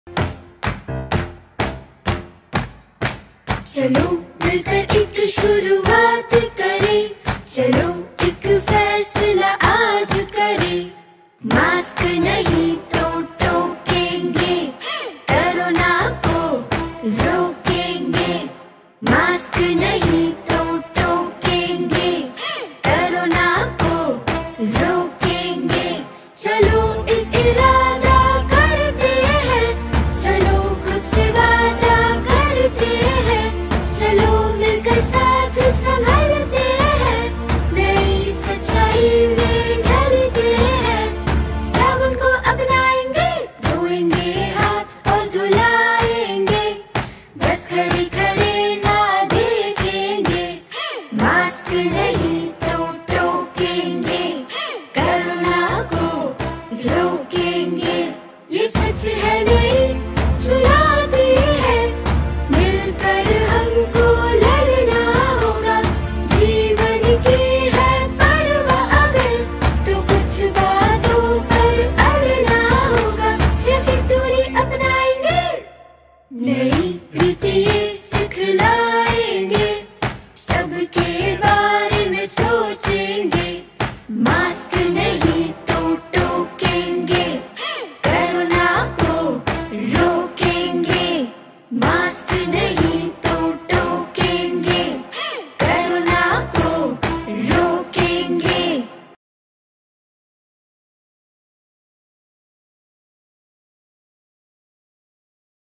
मास्क नहीं तो टोकेंगे कोरोना को रोकेंगे !
मेरा मास्क कोरोना के खिलाफ मेरी ढाल है | कोविड अनुरूप व्यव्हार अपनायें, सुरक्षित रहें।
वैक्सीन की पहली डोज लगवाने के बाद भी मास्क लगाना बेहद जरुरी है इसलिए टीकाकरण के बाद भी कोविड उपयुक्त व्यवहार का पालन करें और अपने तथा अपने परिवार को स्वस्थ रखें।